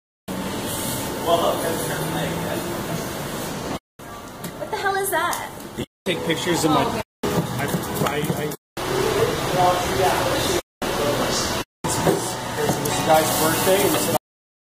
Canvas Underground's Beer Deconstructed Dinner
Peter Jackson and Canvas Underground created a menu using components of the beer making process, including hops,  yeast and malt.